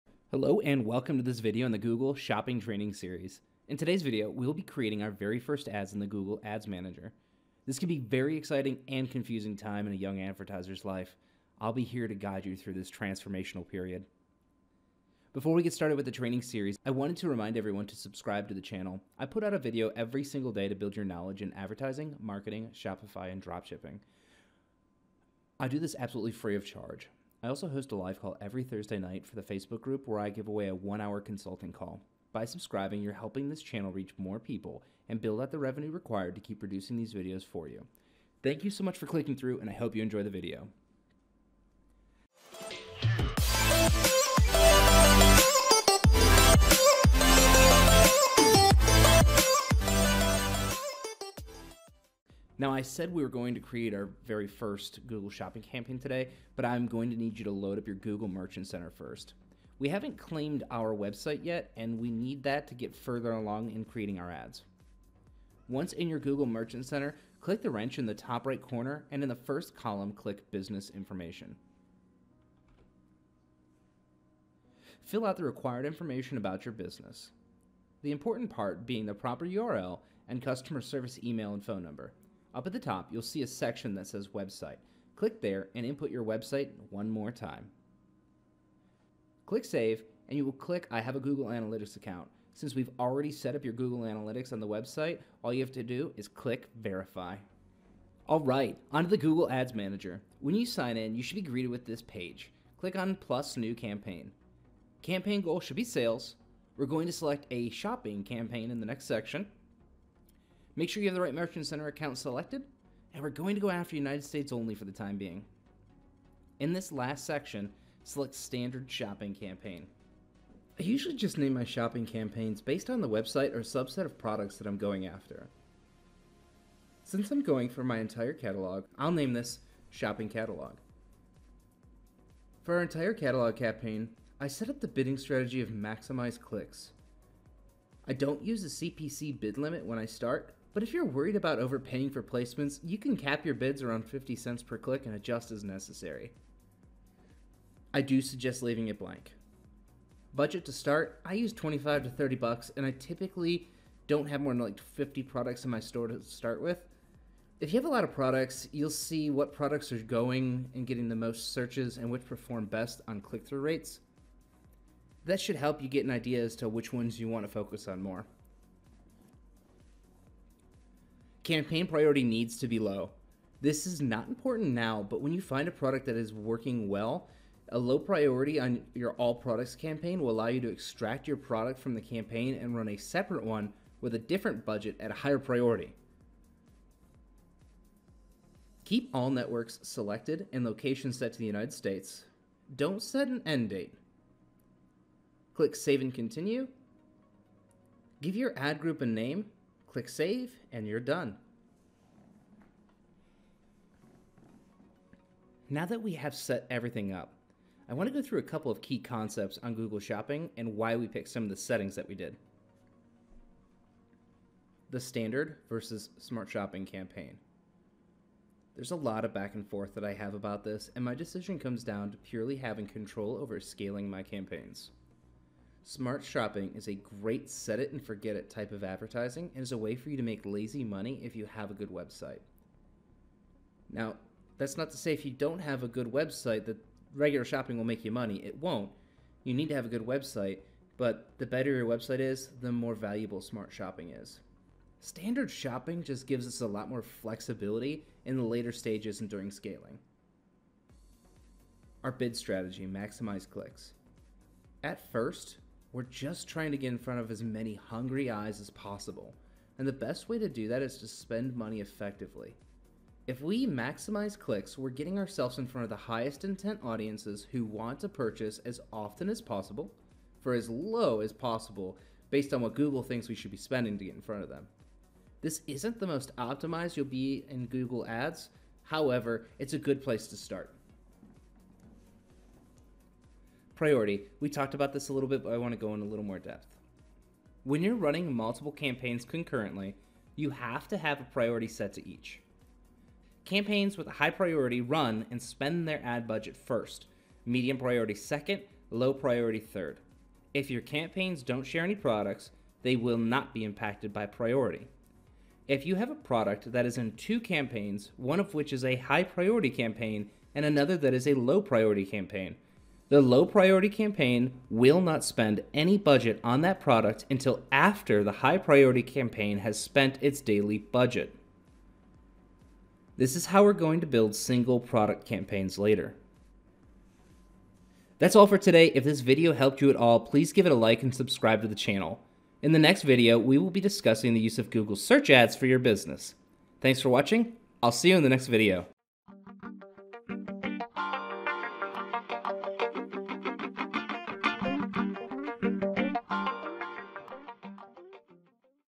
Creating A Google Shopping Campaign - Google Shopping Training Series 2020
Today we get to set up our very first Google Shopping Campaign.  I have been hyping this moment up for you for a few weeks now in the Facebook group, but the truth is... it's really easy!  We did that hard part in the first 3 videos!

Now, I said we were going to create your very first Google Shopping Campaign today, but I am going to need you to load up your Google Merchant center first.  We haven't yet claimed our website and we need to get this together before we create our Ads.  Once in your Google Merchant center, click the wrench in the top right corner and in the first column click business information.  Fill out the required information about your business.  The important part being the proper URL and customer service email and phone number.  Up at the top, you will see a section that says website, click there and input your website one more time.  Click save and then you will click I have a Google Analytics account.  Since we have already setup your Google Analytics on your website all you have to do is click verify!

Alright!  On to Google Ads Manager.  When you sign in you should be greeted with this page, click on +New Campaign.  Campaign Goal should be Sales.  We are going to select a Shopping Campaign in the next section.  Make sure you have the right merchant center account selected and we are going to go after United States only for the time being.  In the last section, select Standard Shopping Campaign.

I usually just name my shopping campaigns based on the website or subset of products that I am going after.  Since I am going after my entire catalog, I will name this Shopping Catalog.  For our entire catalog campaign, I set up the bidding strategy of Maximize clicks.  I do not use a CPC bid limit when I start, but if you are worried about overpaying for placements, you can cap your bids around $0.50 per click and adjust as necessary.  I suggest leaving this blank.  Budget to start, I use $25 and I typically don't have more than 50 products in my store to start with.  If you have a lot of products, you will see what products are getting the most searches and which perform best based on click through rates.  Campaign priority needs to be low.  This is not important now, but when you find a product that is working incredibly well, a low priority on your all products campaign will allow you to extract your product from this campaign and run a separate one with a different budget at a higher priority.  Keep all networks selected and Location set to United states.  Don't set an end date and save and continue.  Give your Ad Group a name, click save and you are done!

Now that we have set everything up, I want to go through a couple of key concepts with Google Shopping and why we picked some of the settings that we did.

The Standard Shopping Campaign Vs Smart Shopping

There is a lot of back and forth that I have about this and my decision comes down to purely having control over scaling my campaigns.  Smart shopping is a great set it and forget it type of advertising and is a way for you to make lazy money if you have a good website.  If you don't have a good website, don't expect to make any money off of either of these options.  Standard shopping just gives a lot more flexibility in the later stages of scaling.

Big Strategy: Maximize Clicks

At first, we are just trying to get in front of as many hungry eyes as possible and the best way to do that is to spend money effectively.  If we maximize clicks we are getting ourselves in front of the highest intent audiences who want to purchase as often as possible for as low as Google thinks we should spend to get in front of them.  This isn't the most optimized you will be in your Google Ads career, but it is a good place to start.

Priority

When you are running multiple campaigns concurrently, you have to have a priority set to each.  Campaigns with a high priority run and spend their ad budget first, medium priority second, low priority third.  If your campaigns don't share any products, they will not be impacted by priority.  If you have a product that is in two campaigns, one of which is a high priority campaign and another that is a low priority campaign.  The low priority campaign will not spend any budget on that product until after the high priority campaign has spent its daily budget.  This is how we are going to build single product campaigns later.